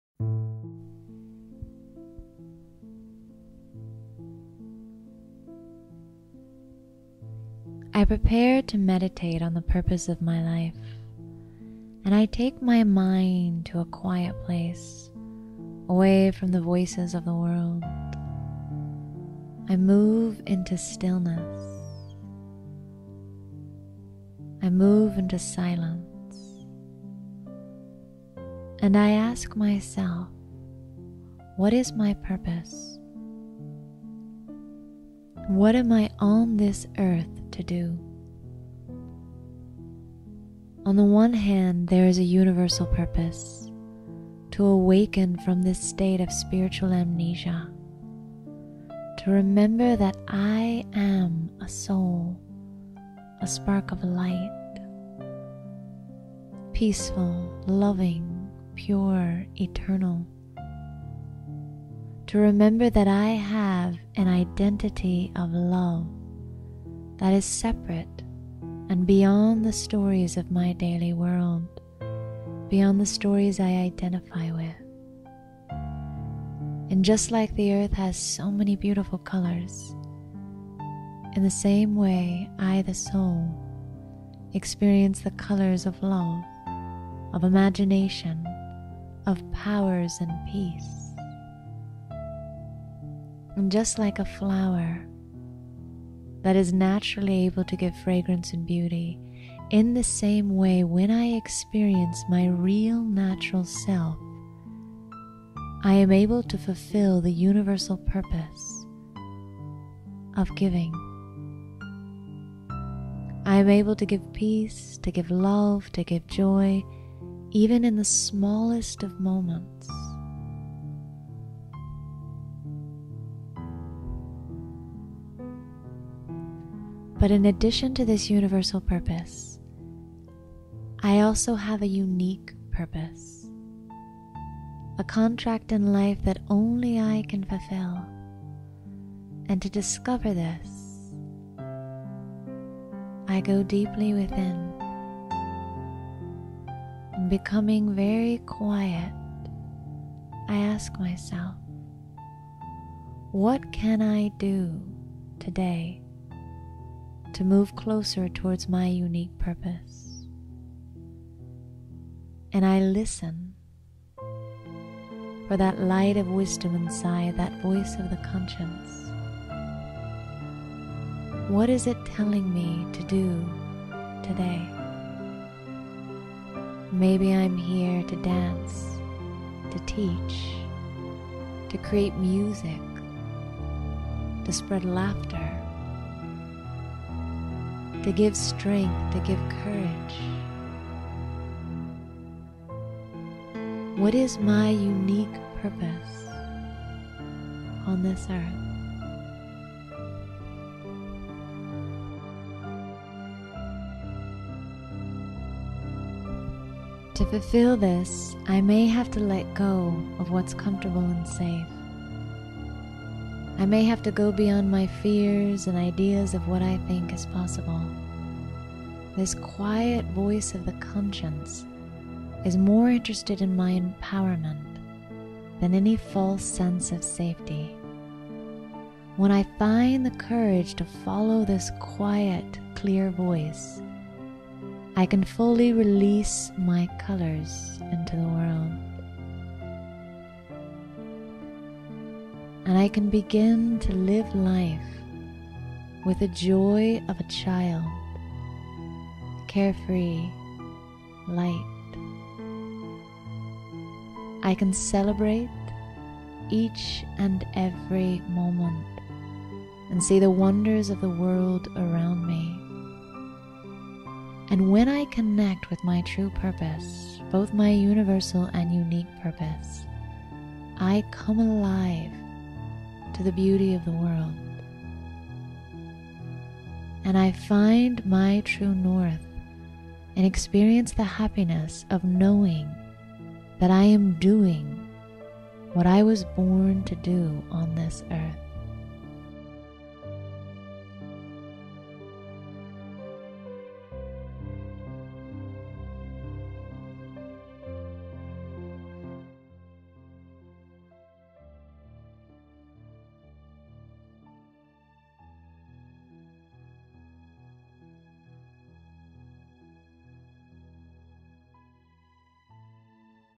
Meditation on Purpose
Purpose- On the one hand we have a universal purpose to awaken spiritually and unfold the colors of our being into the world. But we also have a unique purpose, a contract only we can fulfill. To fulfill that contract, we may have to let go of the familiar and be willing to take risks. This reflection takes you into a few moments of introspection so that you can ask yourself, "What is my unique purpose?"